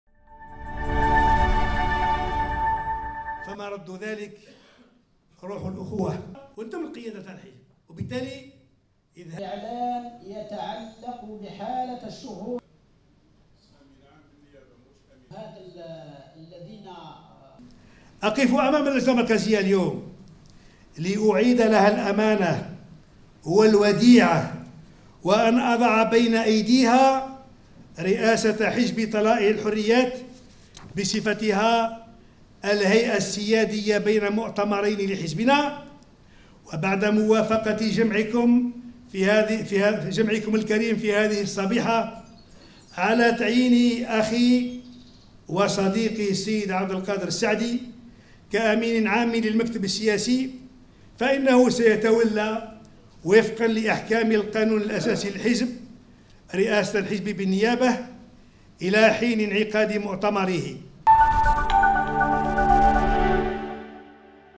رئاسة حزب طلائع الحريات: بن فليس يستقيل وسعدي يخلفه بالنيابة
الجزائر(وأج) - استقال علي بن فليس اليوم رسميا من رئاسة طلائع الحريات ليتولى عبد القادر سعدي إدارة شؤون الحزب بالنيابة لحين انعقاد مؤتمره خلال السداسي الأول من عام 2020.